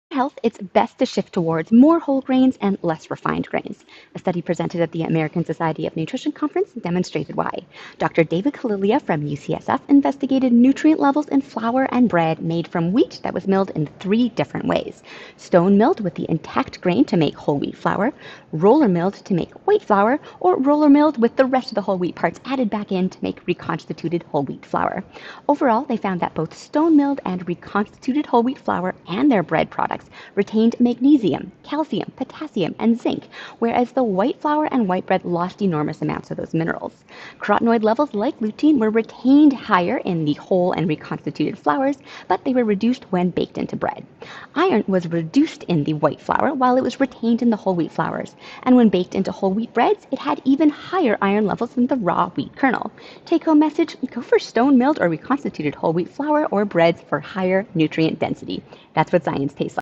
Whole wheat flour is more nutrient dense
For health it’s best to shift towards more whole grains and less refined grains. A study presented at the American Society of Nutrition conference demonstrated why. Dr. David Killilea from UCSF investigated nutrient levels in flour and bread made wheat that was milled in three different ways: stone milled with the intact grain to make whole wheat flour, roller milled to make white flour, or roller milled to with the rest of the whole wheat parts added back to make reconstituted whole wheat flour. Overall, they found both stone milled and reconstituted whole wheat flour and their bread products retained magnesium, calcium, potassium and zinc, whereas the white flour and white bread lost enormous amounts of these minerals. Carotenoid levels like lutein were retained higher in the whole and reconstituted flours, but they were reduced when baked into bread. Iron was reduced in white flour while it was retained in the whole wheat flours, and when baked the whole wheat breads had even higher iron than the raw wheat kernel. Take home message: go for stone milled or reconstituted whole wheat flour or breads for higher nutrient density. That’s what science tastes like.

Follow Nourishable to stay up to date on all things nutrition.